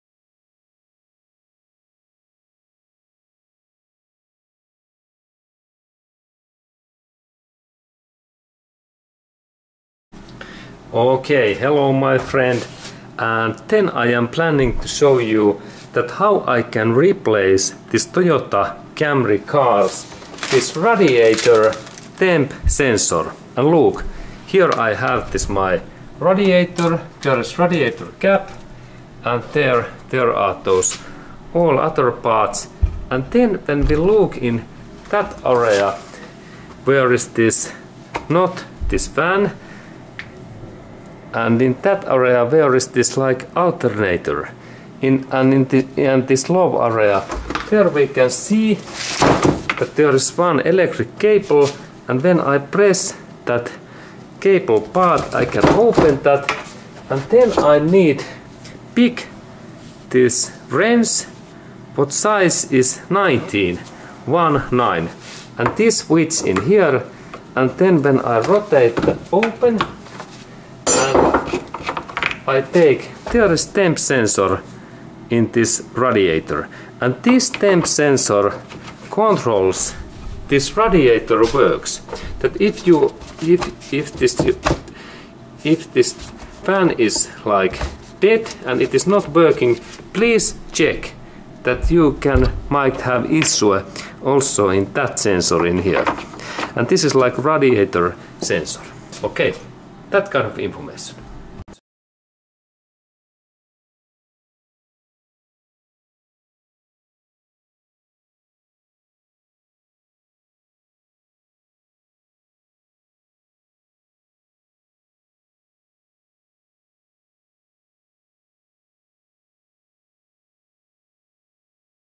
How to replace radiator temp sensor Toyota Camry Automatic transmission. Years 1991 to 2017
How to replace radiator temp sensor Toyota Camry. Years 1991 to 2017